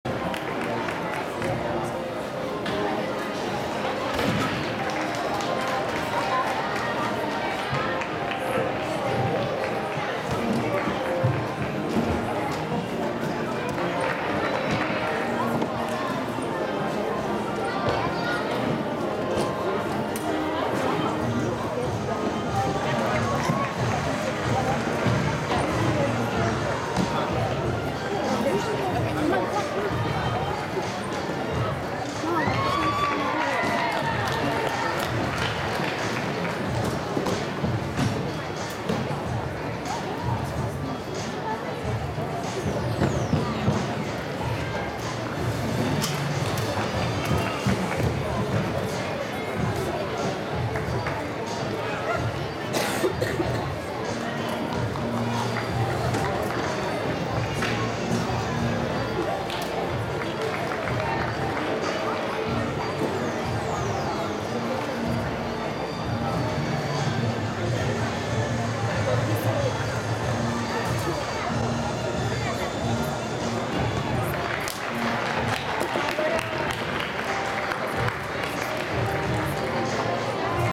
Leah Clapper Beam - 2015 Elite Qualifier Columbus
Level: Jr. Elite
Session 2 of the National Elite Qualifier, recorded on February 6, 2015, in the Celeste Building of the Ohio Expo Center in Columbus, OH.